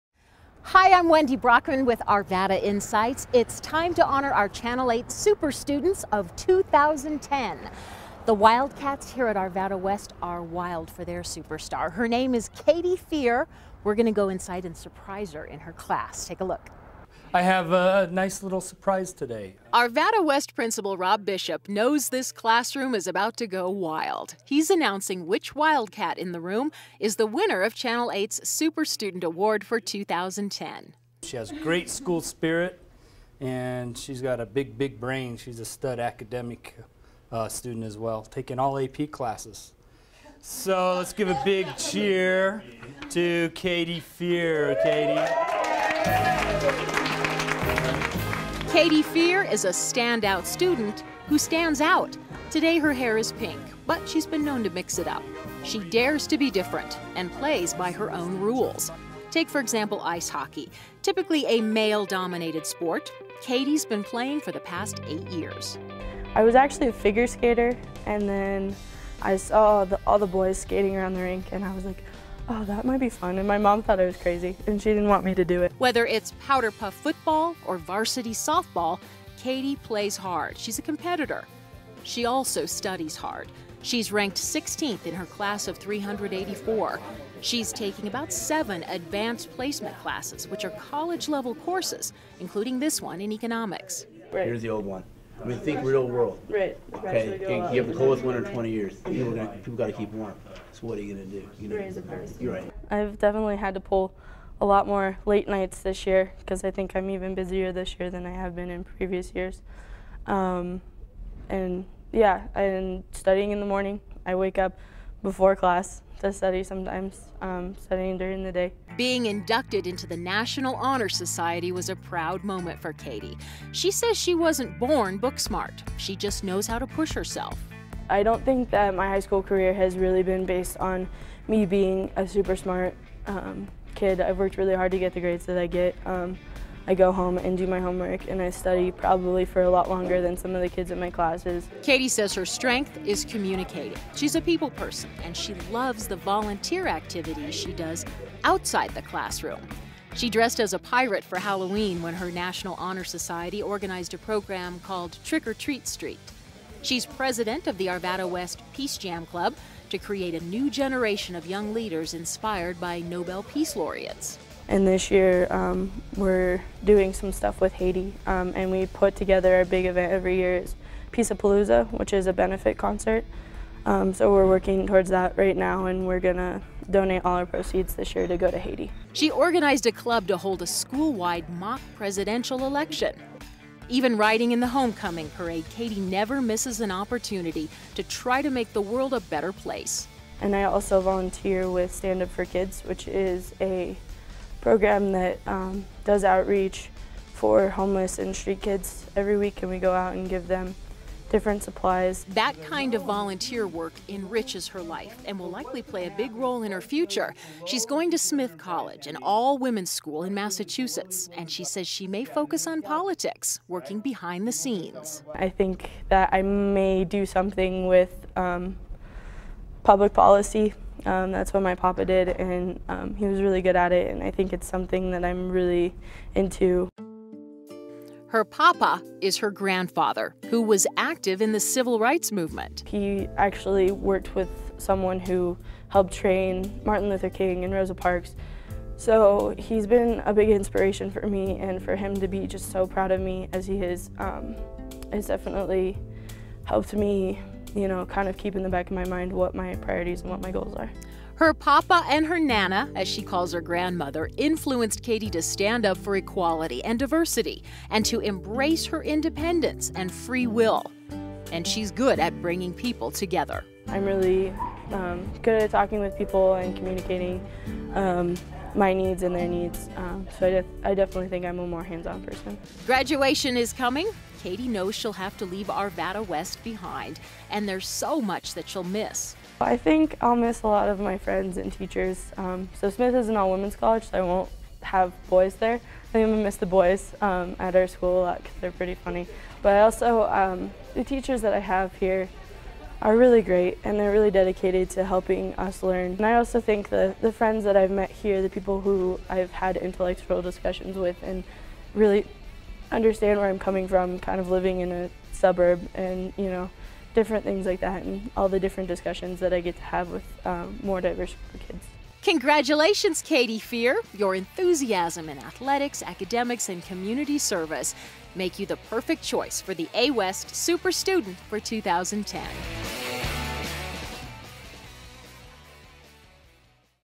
Arvada Insights - Super Student Katie Fear.m4v
Arvada Insights introduces it's four "Super Students" of the year.  Meet Katie Fear from Arvada West High School and see why she was picked.